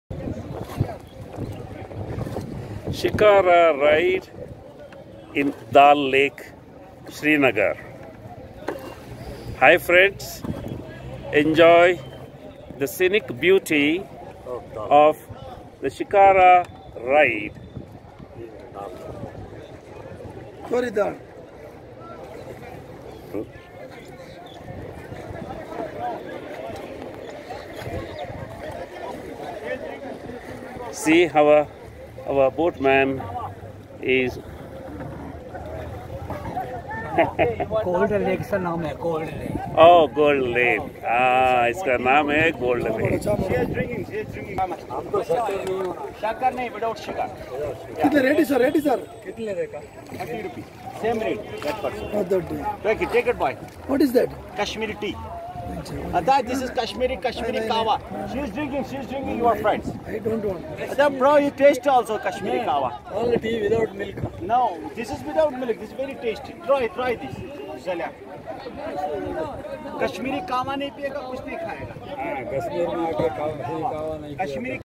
Enjoying the Shikhara ride in Dal Lake, Srinagar 16th April 2021
Shikhara ride in Dal Lake